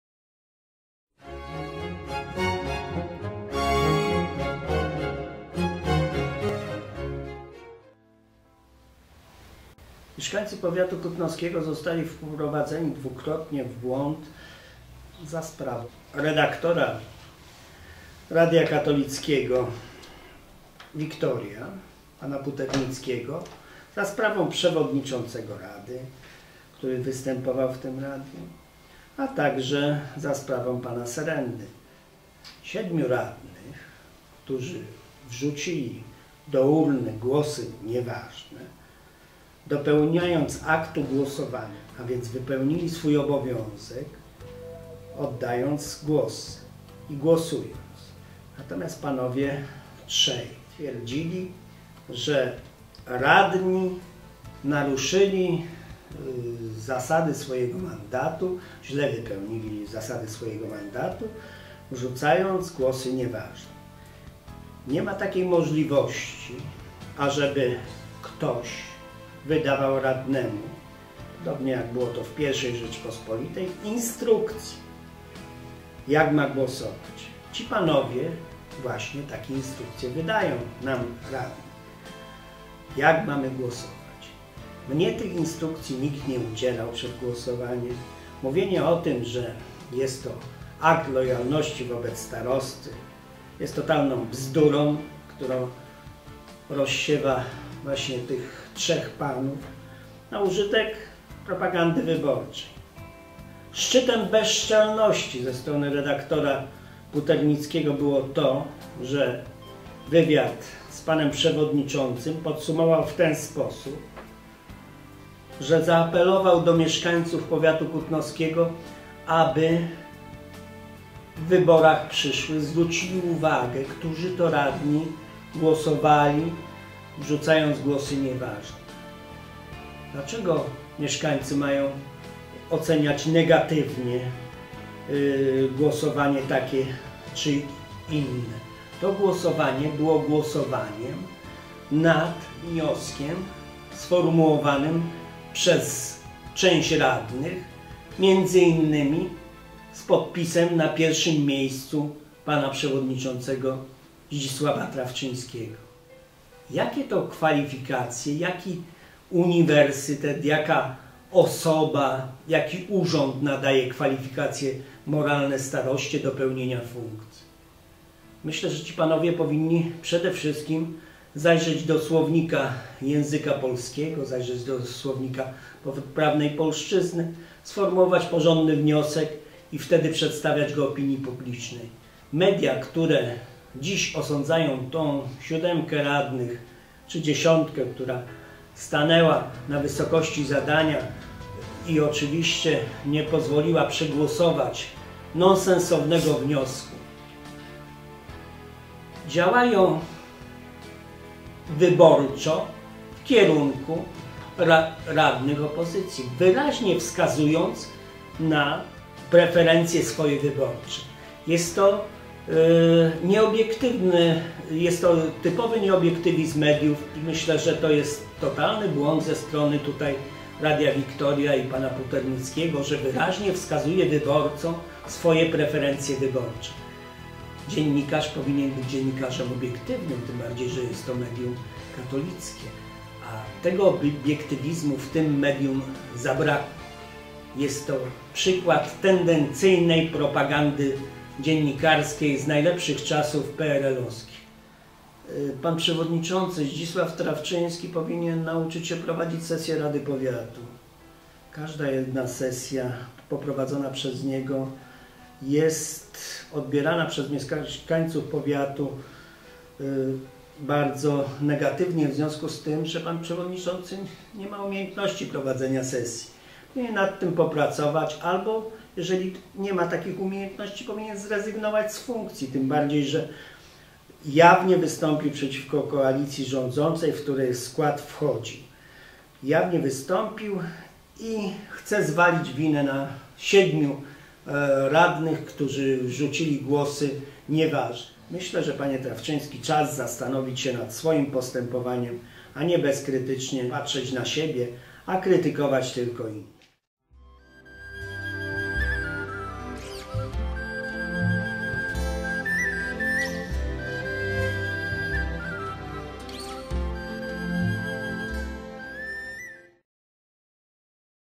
Jerzy Pawlak mówi o nierzetelnej informacji
Członek Zarządu Starostwa Powiatowego w Kutnie odnosi się min. do informacji zaprezentowanych przez jednego z dziennikarzy lokalnej rozgłośni radiowej